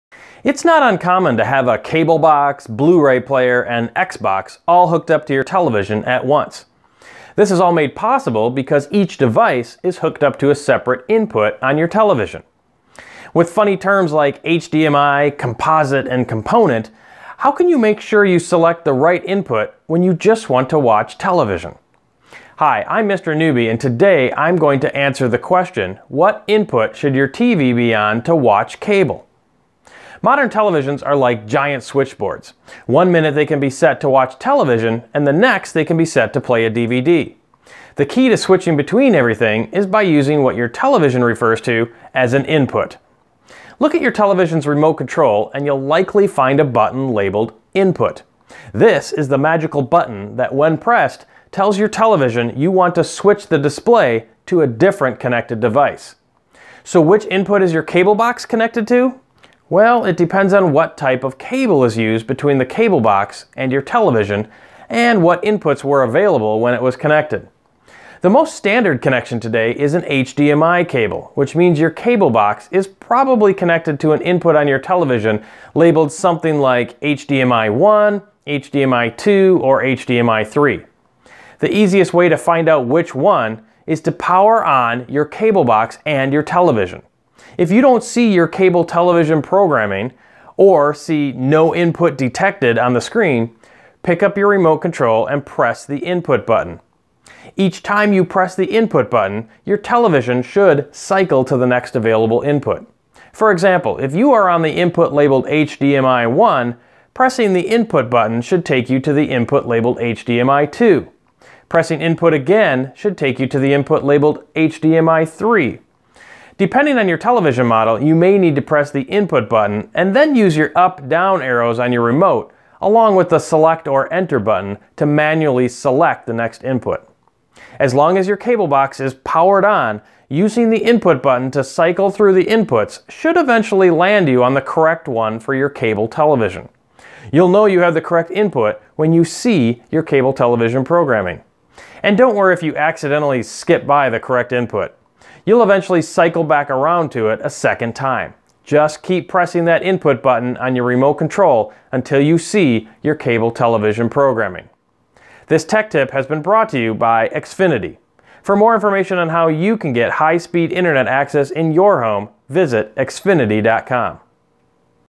What input should your TV be on to watch cable?
Modern televisions are like giant switchboards; one minute it's set to watch TV and the next it can be set to play a video game. Learn how to select the right input to watch cable with this video from Mr. Noobie.